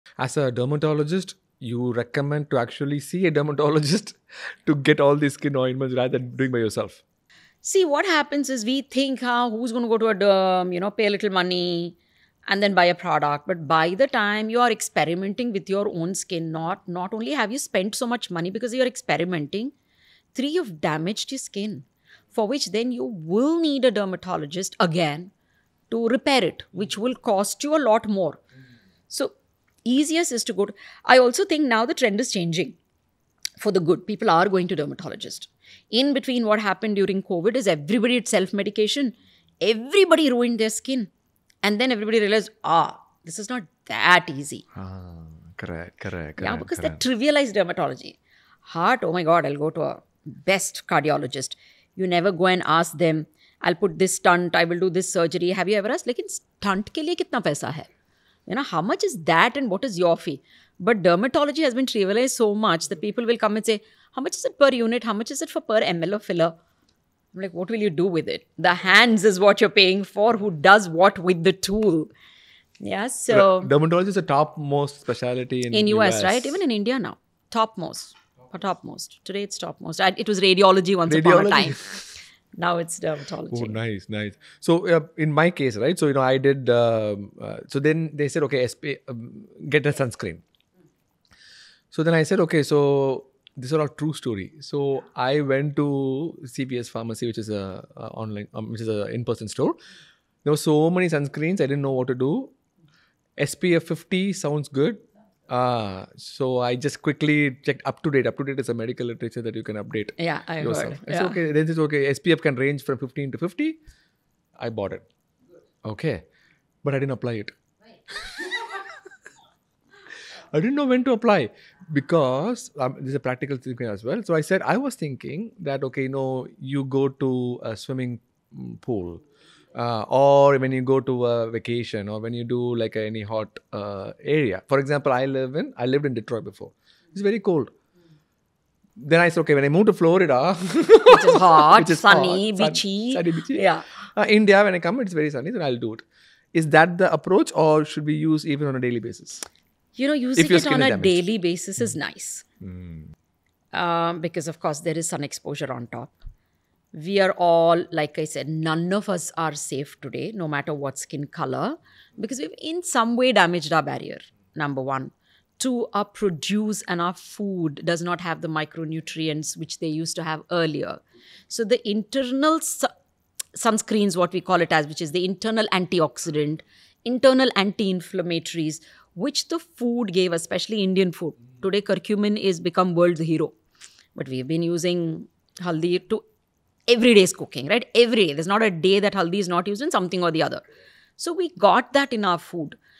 Self-Care Skincare Routine vs. Professional Help from a Dermatologist: Which Is the Right Approach?
The Gut-Skin Connection: How to Fix Your Skin from the Inside Out! No. 1 Dermatologist, Dr. Rashmi